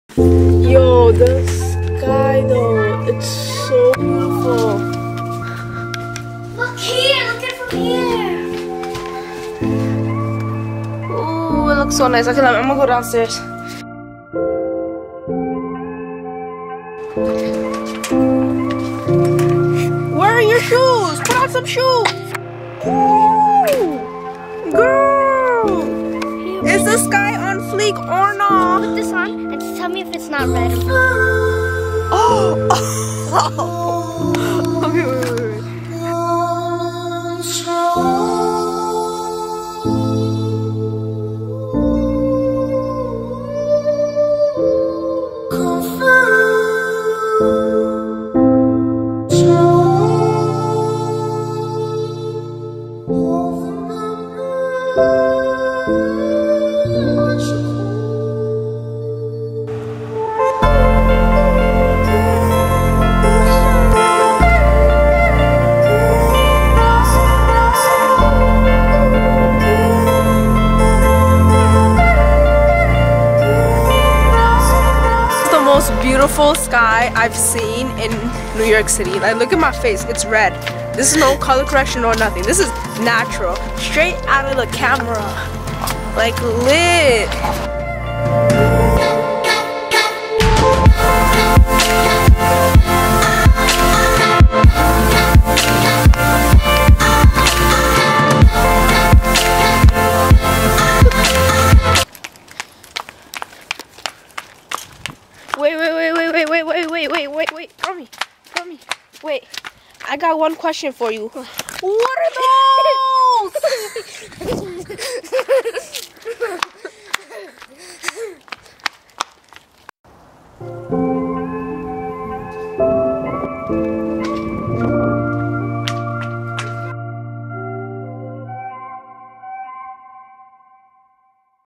INSANE NEW YORK SUNSET
This is probably the most beautiful sunset I have ever witnessed in New York.

Previous Vlog (I MADE IT ON NYC's SNAPCHAT STORY/ Breast Cancer Walk!)